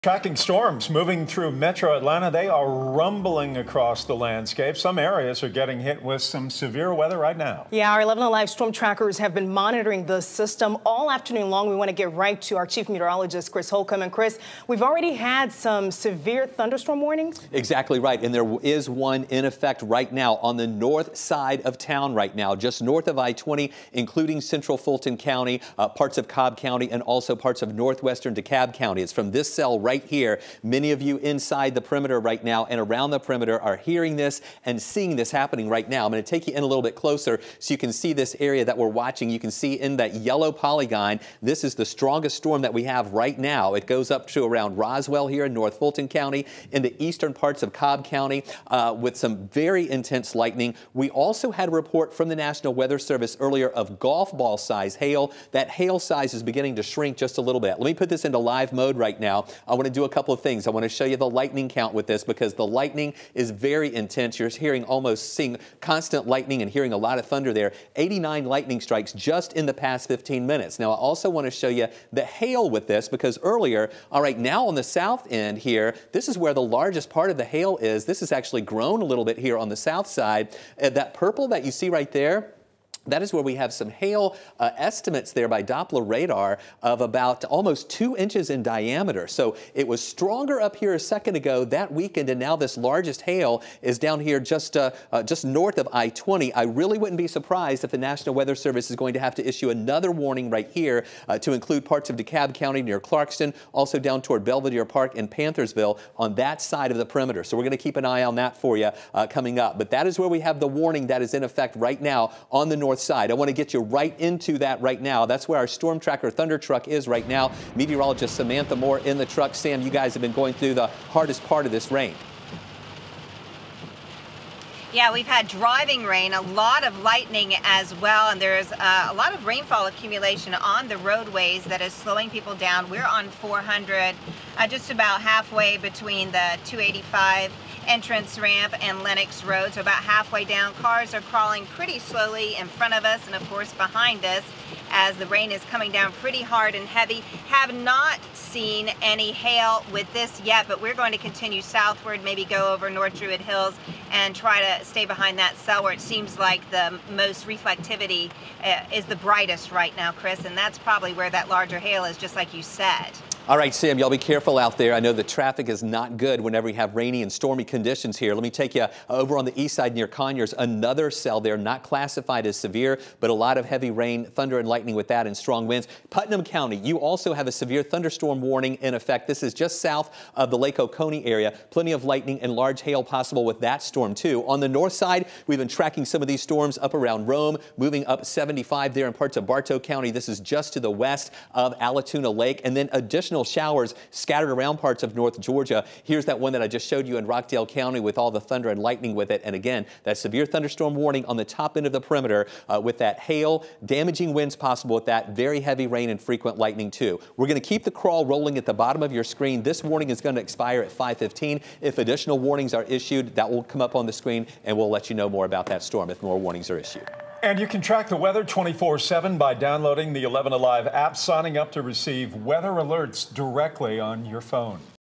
Tracking storms moving through metro Atlanta
Here's an update with the latest severe thunderstorm warnings.